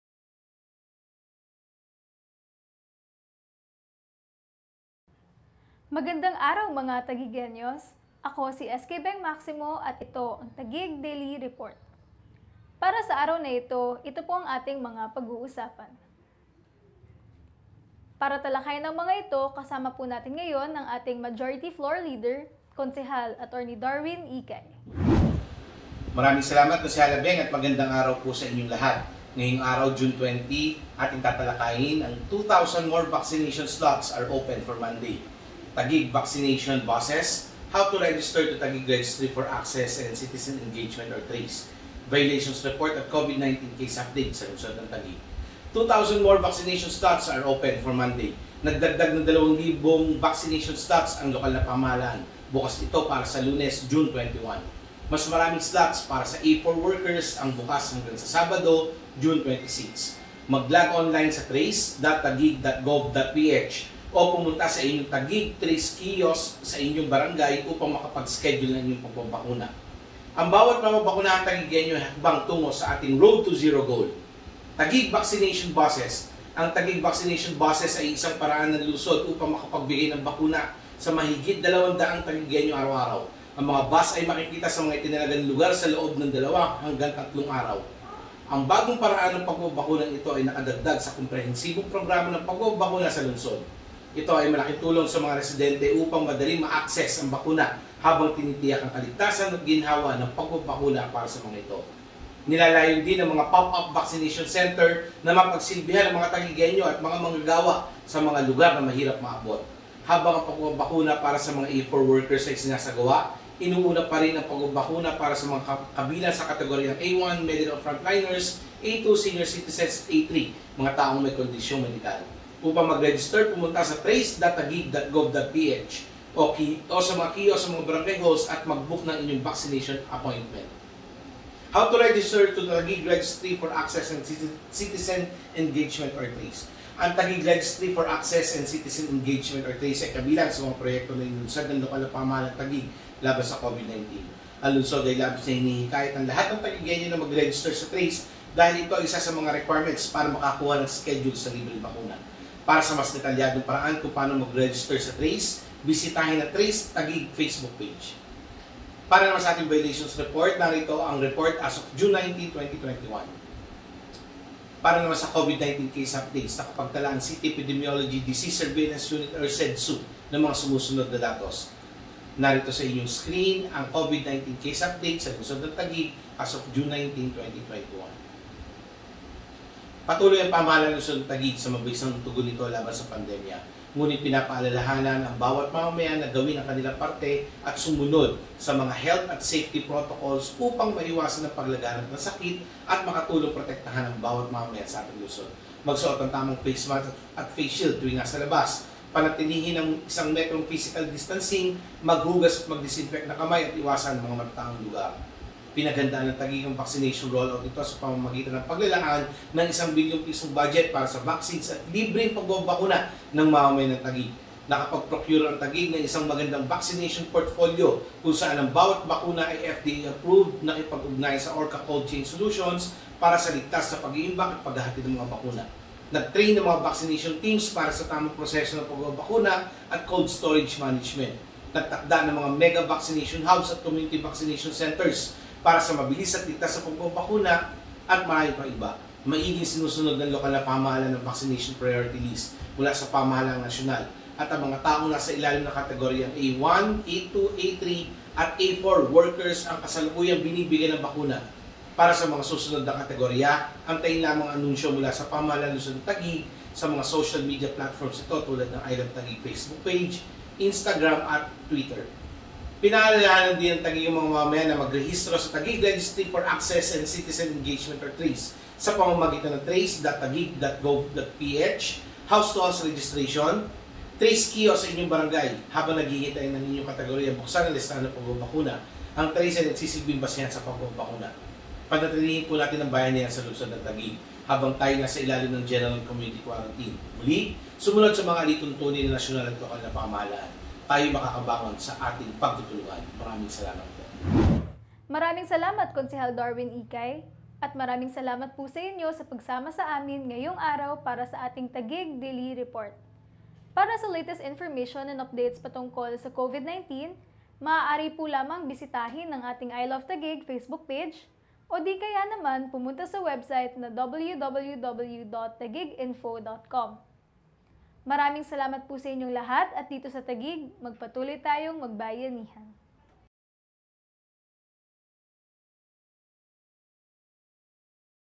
TAGUIG CITY COVID-19 BULLETIN - JUNE 20, 2021
WATCH: Ngayong araw pakinggan ang mga sumusunod na anunsyo o updates ng Lungsod ng Taguig mula kay Councilor Atty. Darwin Icay at SK Federation Chair Beng Maximo:
• 2000 More Vaccination Slots Are Open on Monday
• Taguig’s Vaccination Buses
• How to Register to Taguig Registry for Access and Citizen Engagement (TRACE)
• Violations Report
• COVID-19 Case Updates